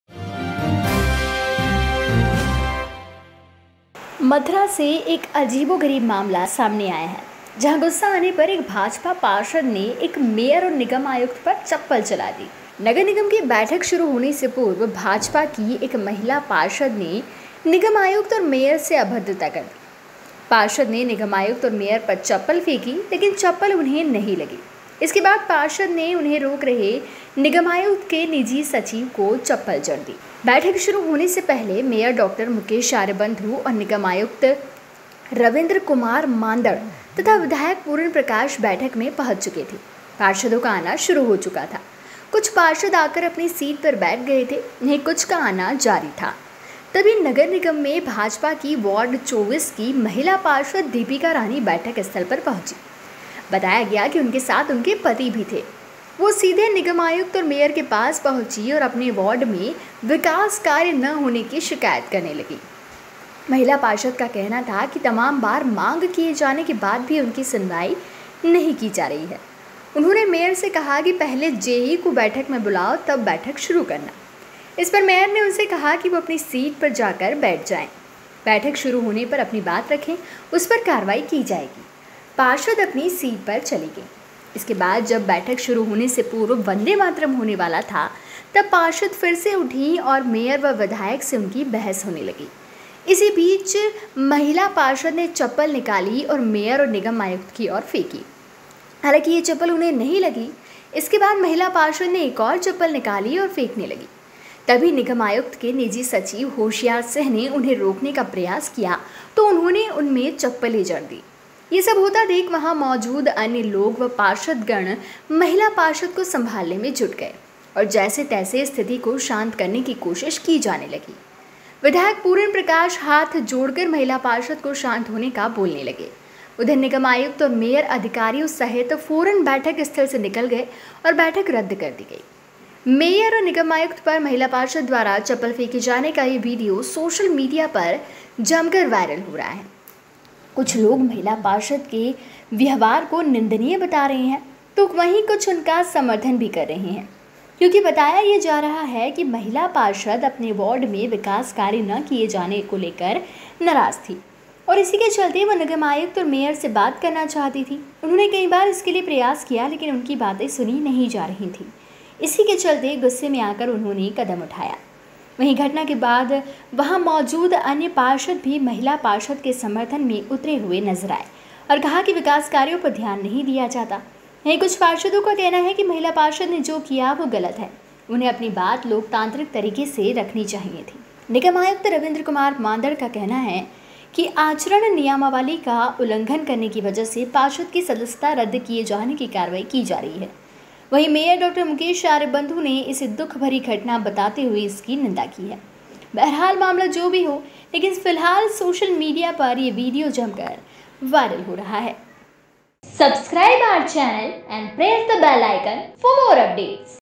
Viral video : नाराज BJP महिला Councilor ने Mayor पर चलाई चप्पल
वार्ड 24 की पार्षद है दीपिका रानी
नगर निगम की बैठक में हुआ हंगामा